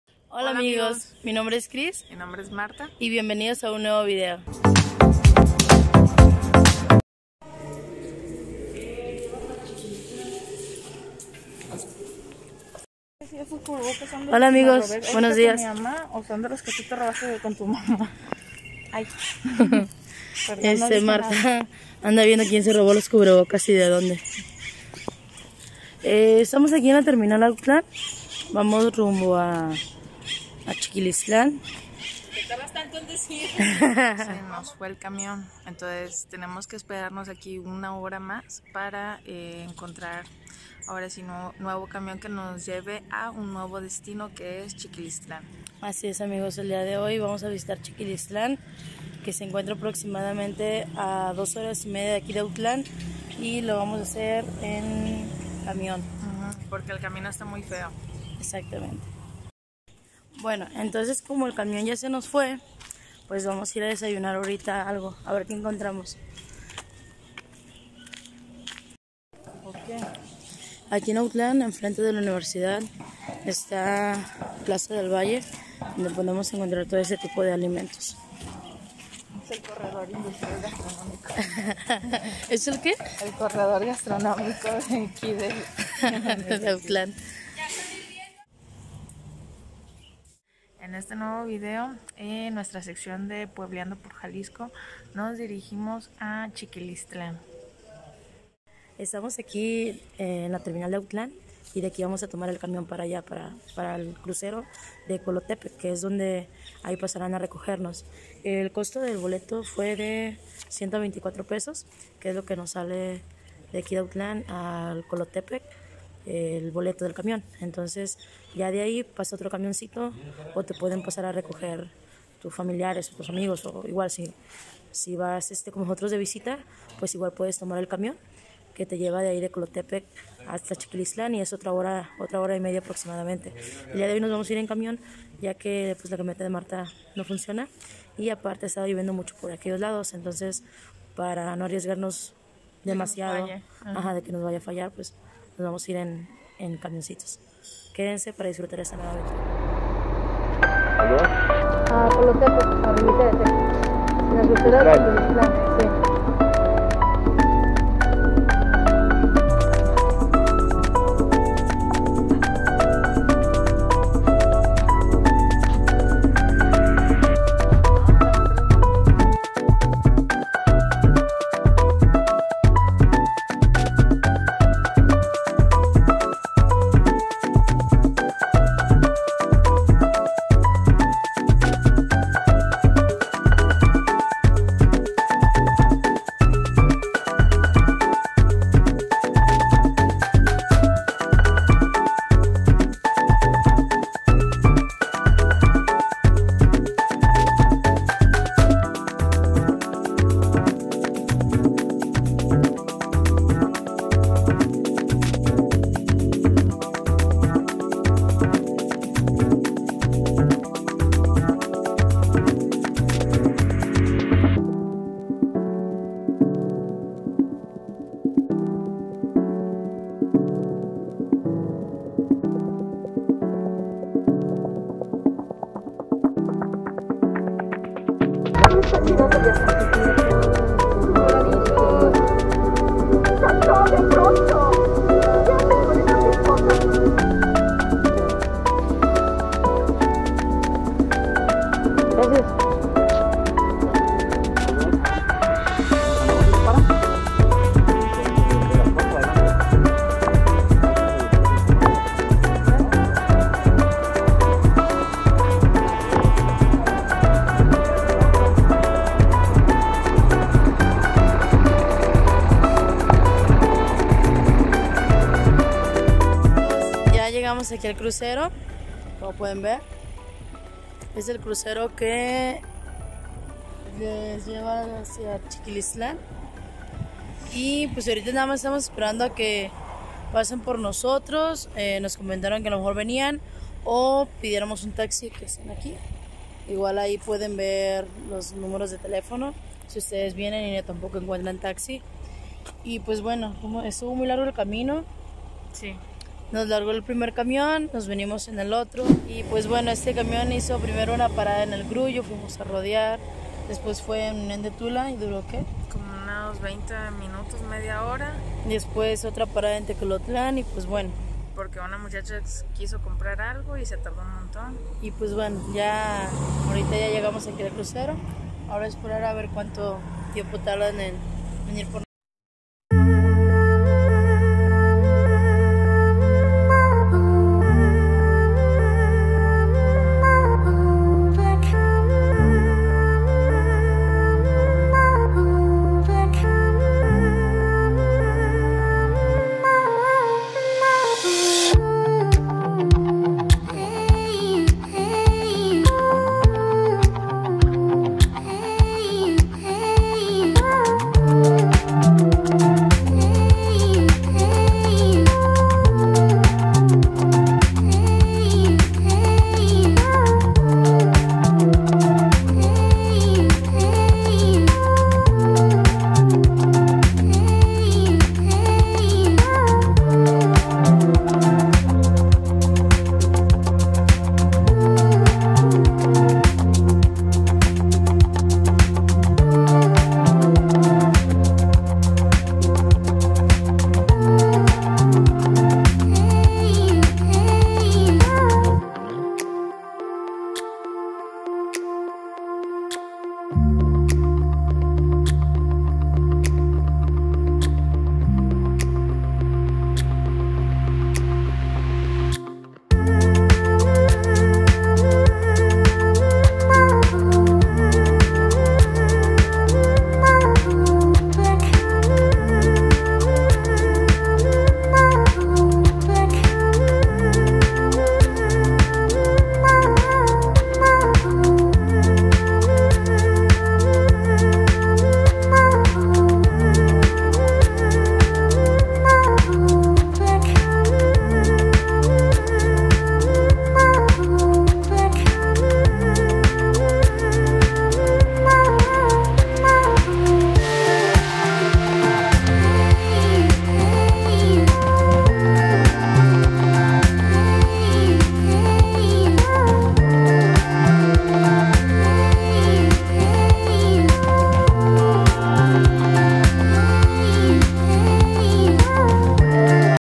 RECORRIENDO CHIQUILISTLÁN | PRIMERA PARTE
Hola amigos en esta sección de recorriendo pueblitos de Jalisco nos fuimos a chiquilistlán, que significa lugar de cirragas.. Es un pueblito pintoresco y muy tranquilo el cual podrás disfrutar de sus calles, de su centro, de sus cascadas..
Ven y disfruta de estos pueblitos de Jalisco.

El camión del crucero de colotepe sale aproximadamente a las 12:30 pm y ahorita solo es una corrida y tiene un costo de 50 pesos. Y el camión de chiquilistlán al crucero sale a las 8 am enfrente de la plaza principal..

También hay lugares de hospedaje y el costo promedio ronda los 600 pesos por noche..